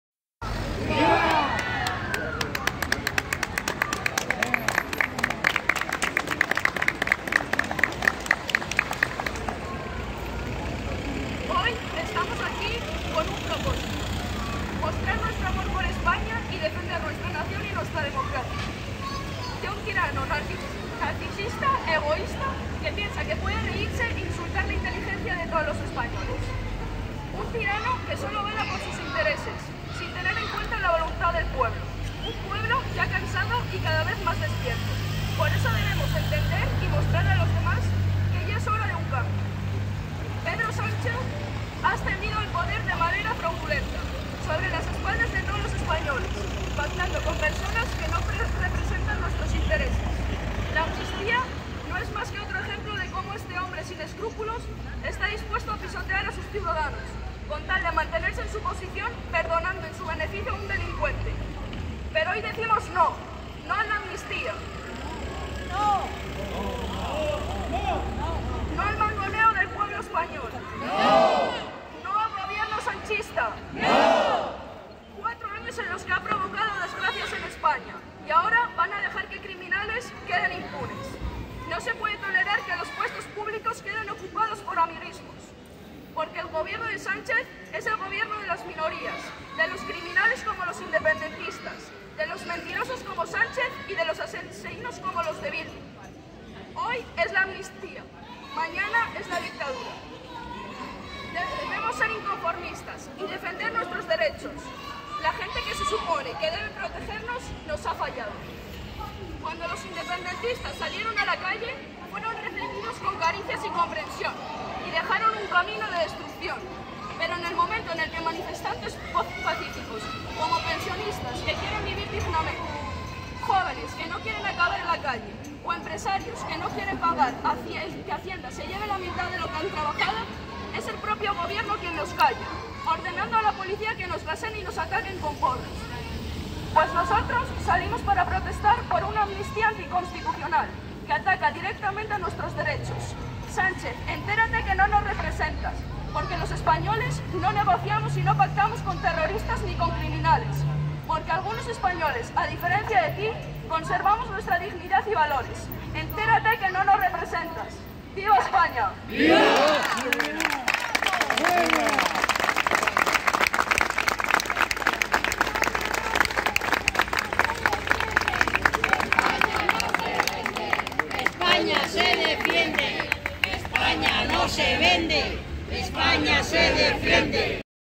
Manifestaciones contra la amnistía (2)
Informativo local
Durante varias noches seguidas gente ha salido a la Plaza Mayor de nuestra ciudad a quejarse de las políticas y actuaciones del Presidente de Gobierno Pedro Sánchez